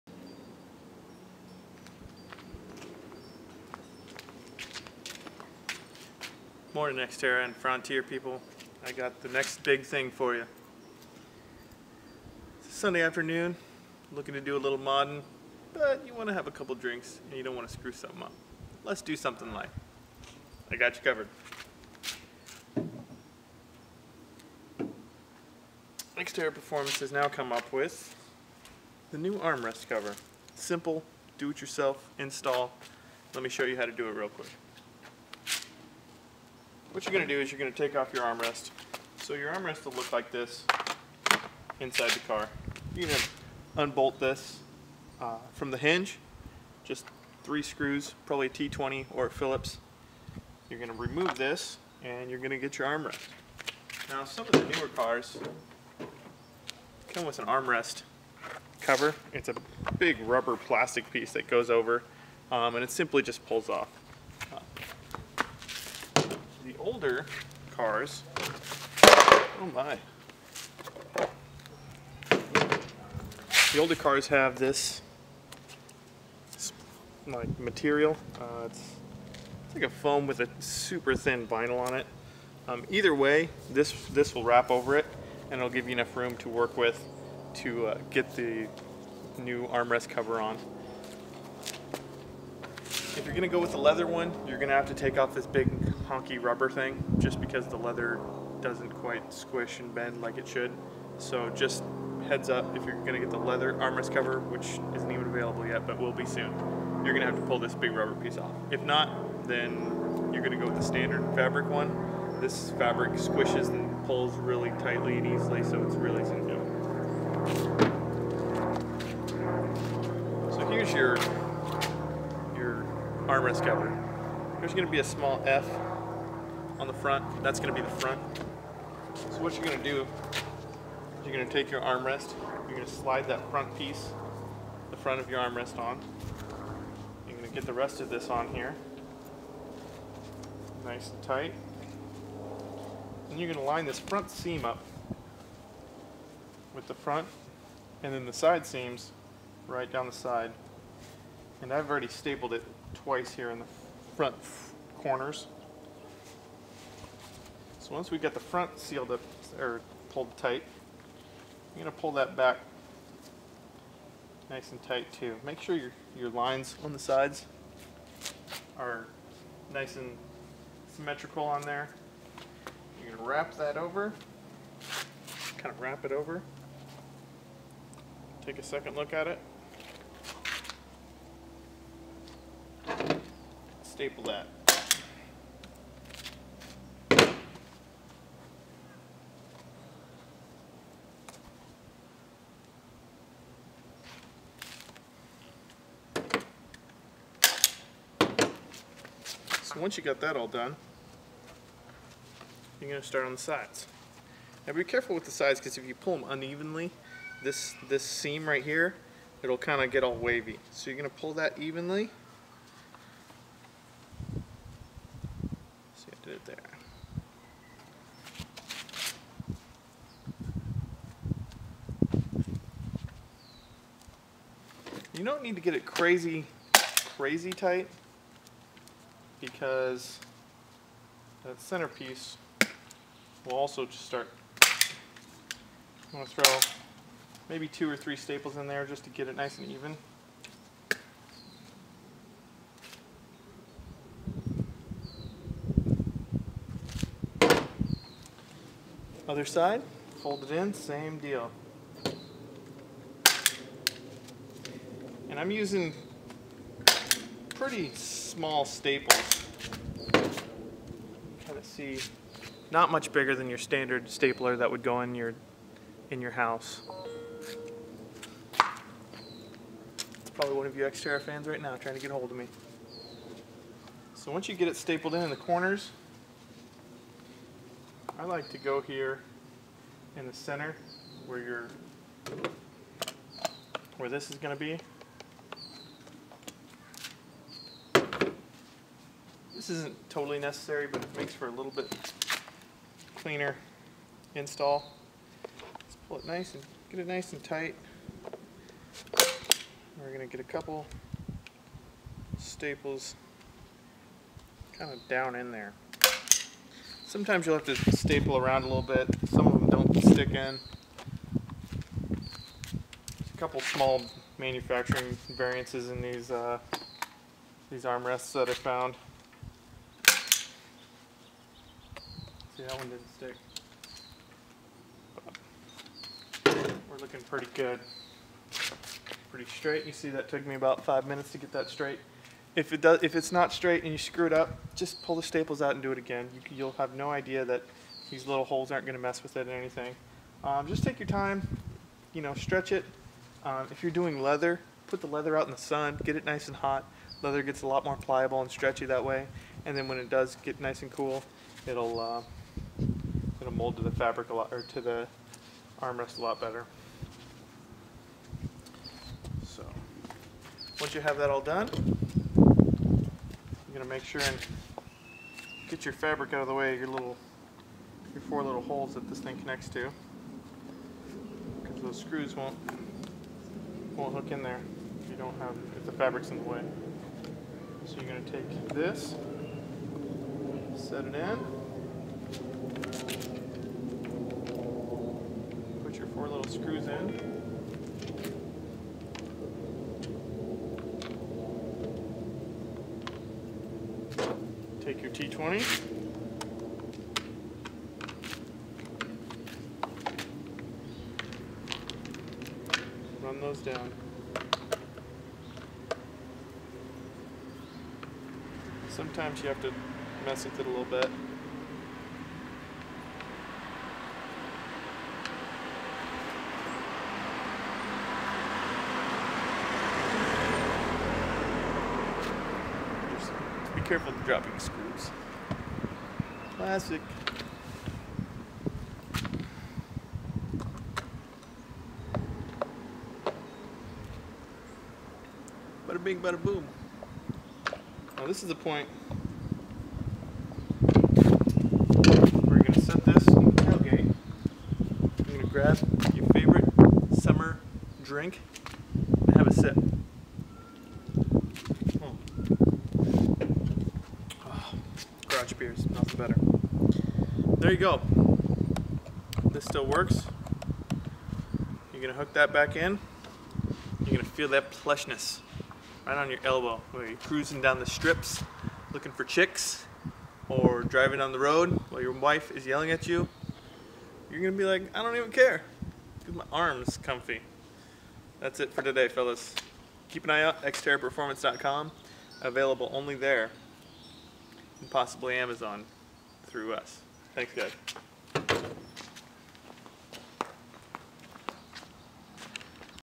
How to Nissan Xterra/Frontier arm rest cover install.
This is how to install the New Center Arm Rest cover for your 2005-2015 Nissan Frontier/Xterra